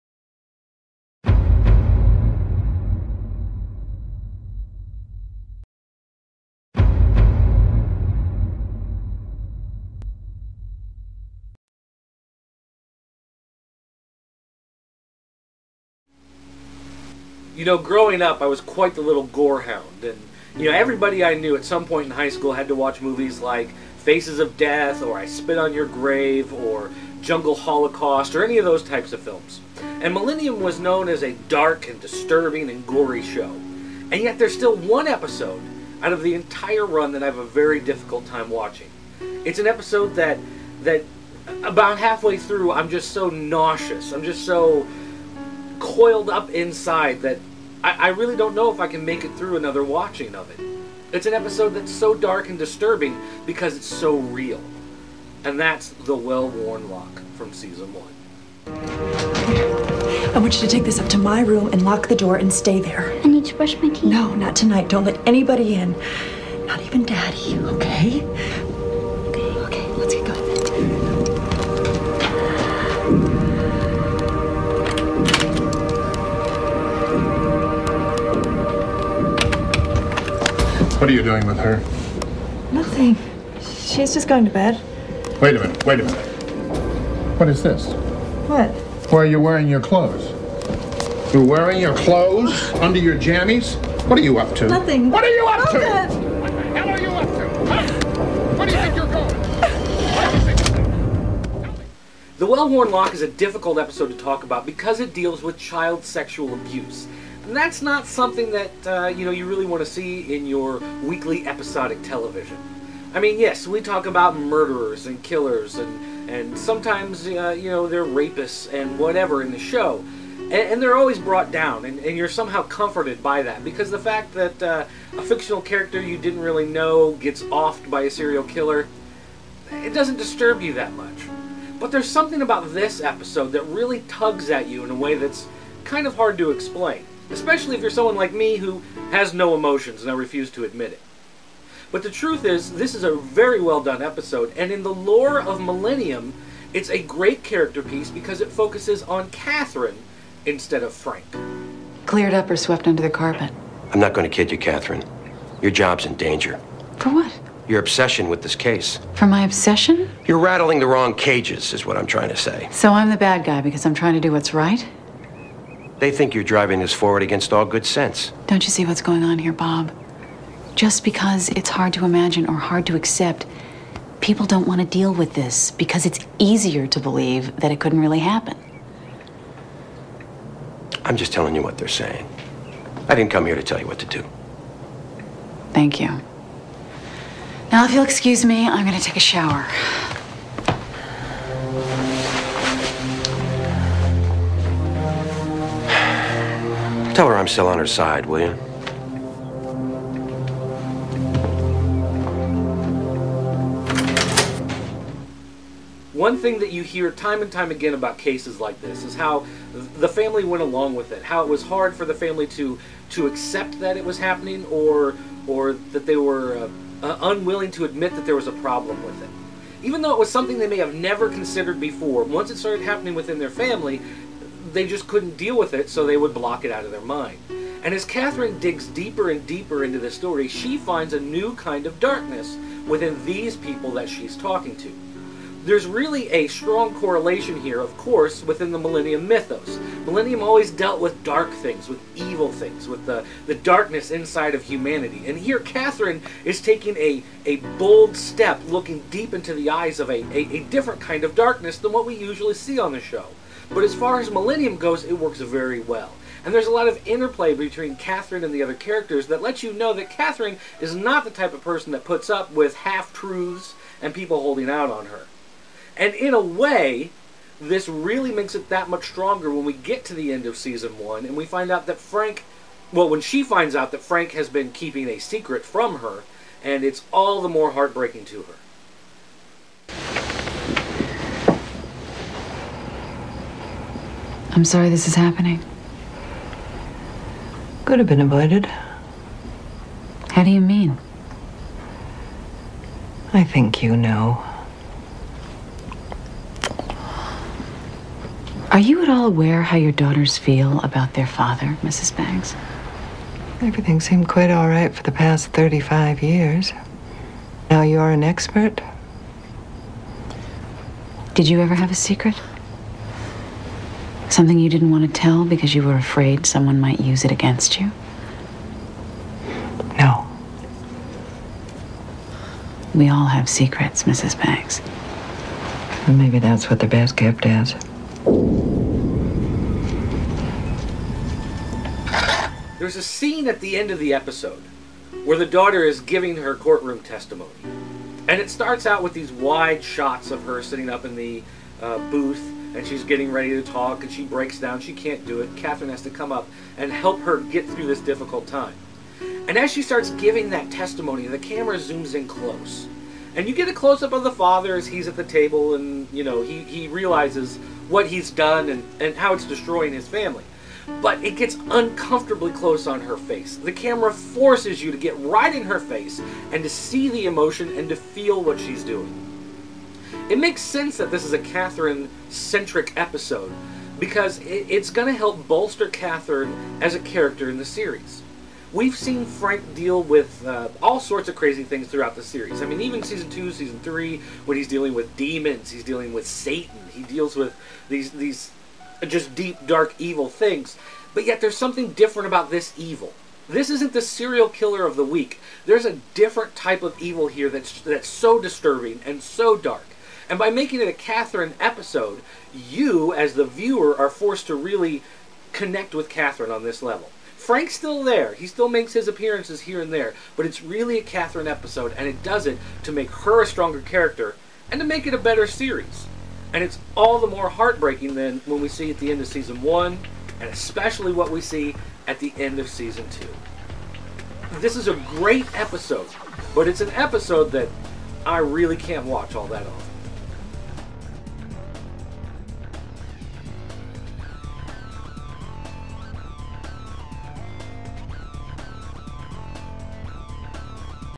MillenniuM Epsiode 108 - The Well-Worn Lock
Creepy Old Dudes Are No Good. Millennium has some creepy episodes to talk about, but none worse than Season 1's "The Well Worn Lock"